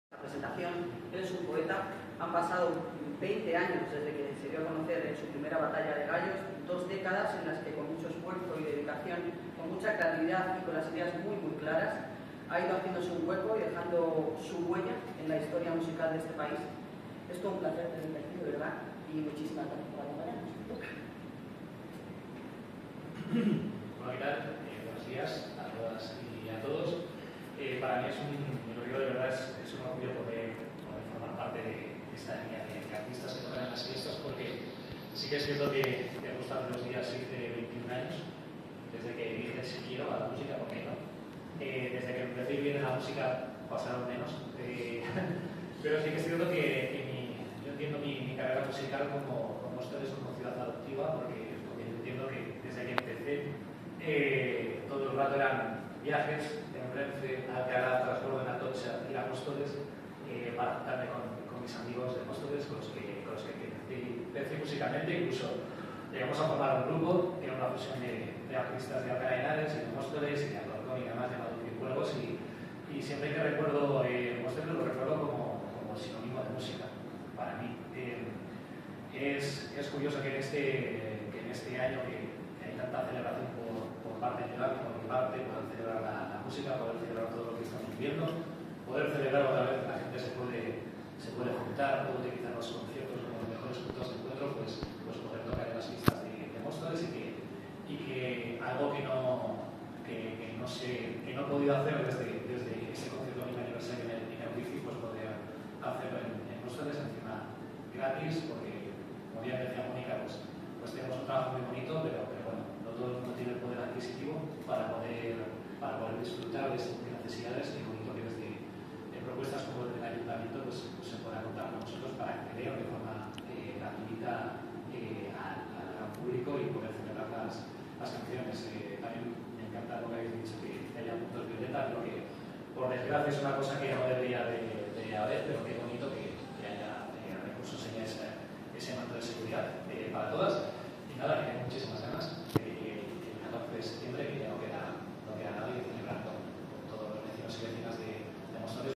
Rayden ,rapero madrileño en la presentación de las fiestas patronales de Móstoles
Rayden ,rapero con 21 años de brillante trayectoria,poeta,y reivindicativo en sus letras ha estado acompañando al equipo de gobierno en la presentación de las fiestas de Móstoles,en honor a nuestra señora de los Santos.podreis verlo el día 14 de septiembre en el escenario de Finca Liana